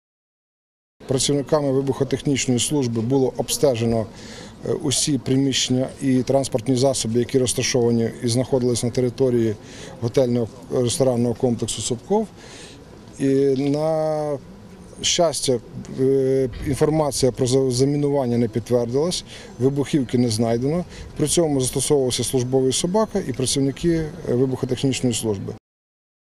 У «Собкоффі» шукали вибухівку
У Хмельницькому замінували готельно-ресторанний комплекс «Собкофф». Таке повідомлення  отримали місцеві правоохоронці. Шукати вибухівку виїхали слідчо-оперативна група, спеціалісти- вибухотехніки та інші екстрені служби. Територію закладу оточили, евакуювали відвідувачів та персонал. .На той час у приміщенні перебувало близько 7 десятків людей. Після кількагодинних пошуків вибухівку у закладі так і не знайшли.